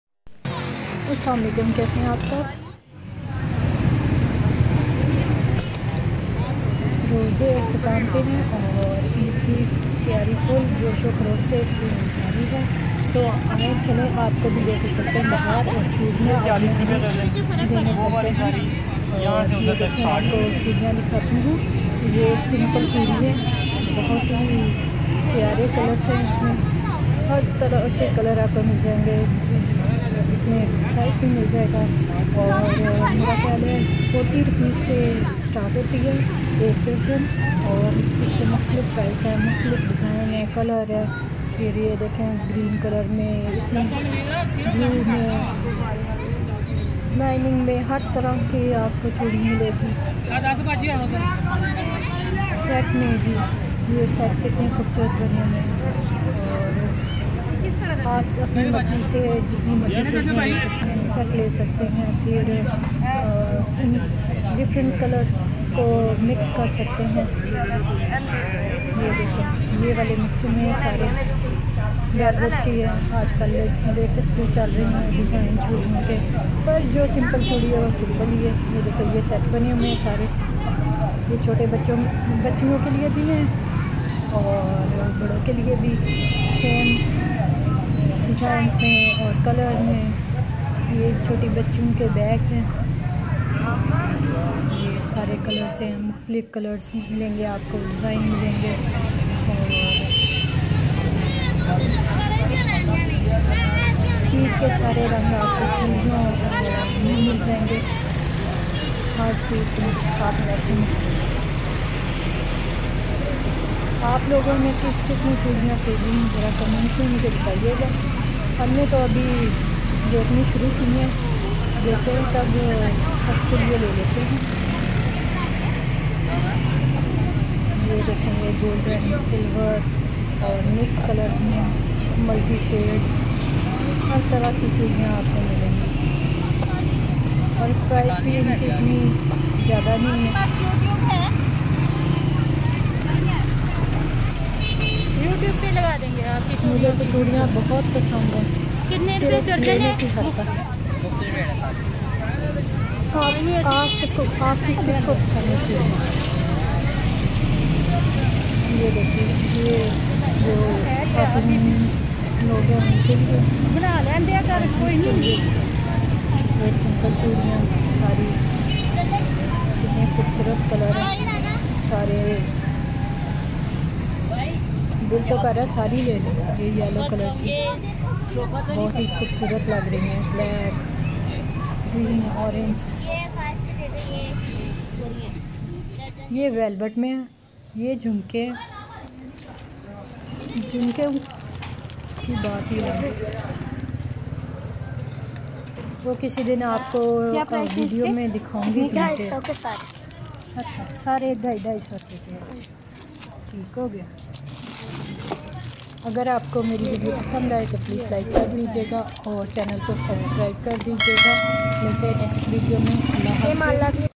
Eid shopping specially Bangals & MEHNDI
A short shopping Vlog on Local Market. Eid shopping specially choriya, MEHNDI, nail paints, dupatta, bags etc...

Hope you ll enjoy shopping with me.                 Your support is greatly appreciated.